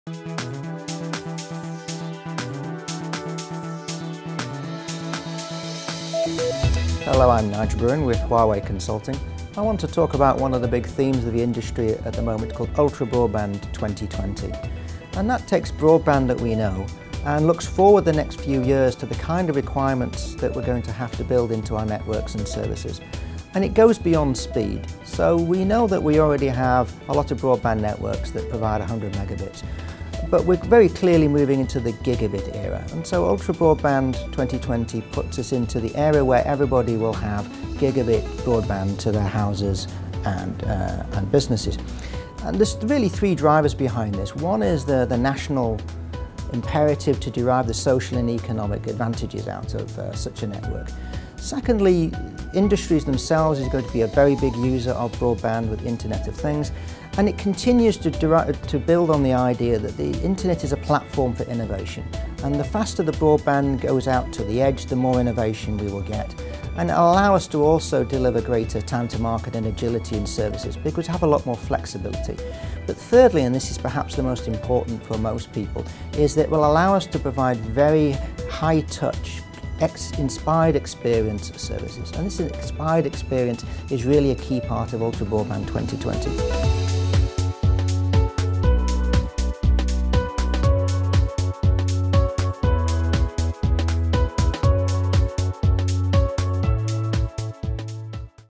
Nigel Bruin - Ultra-Broadband 2020, A Journey to Better Connection [Part 1]
Ultra Broadband 2020 puts us into the era where everybody will have gigabit broadband to their houses and businesses, and there are 3 drivers behind it. Let's watch Nigel's video to understand more.